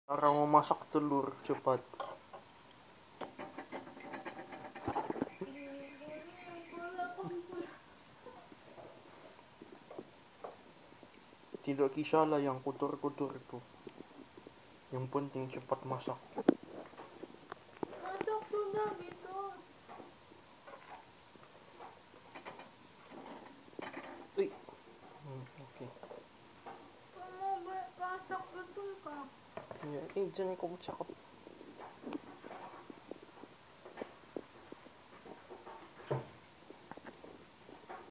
Cara memasak telur tanpa air Dan minyak.
Cara ini sangat berkesan. Tapi awas telur meletup nanti. Ini adalah cara memasak telur yang tidak berapa. Tidak berapa betul. Jadi silakan jangan membuat perkara ni.